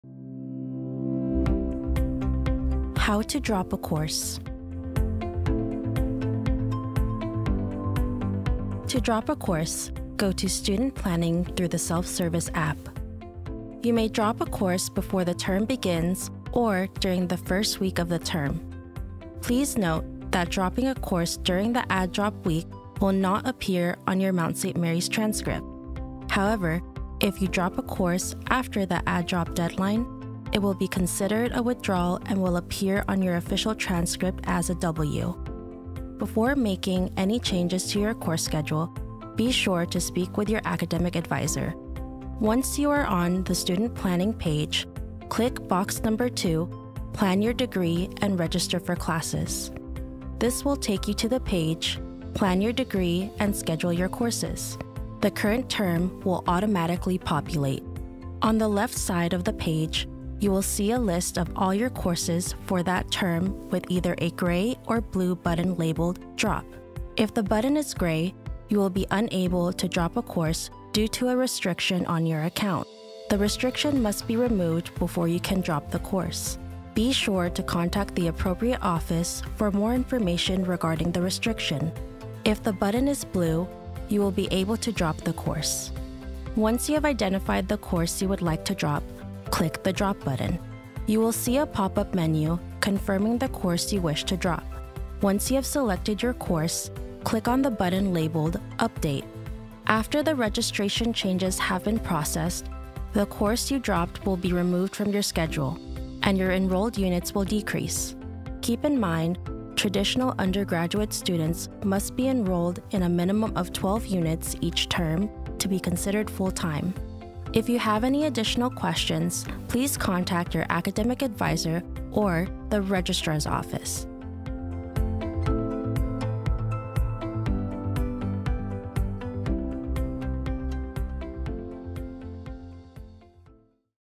Dropping a Course
Step-by-step guide on how to drop a course online through Self-Service.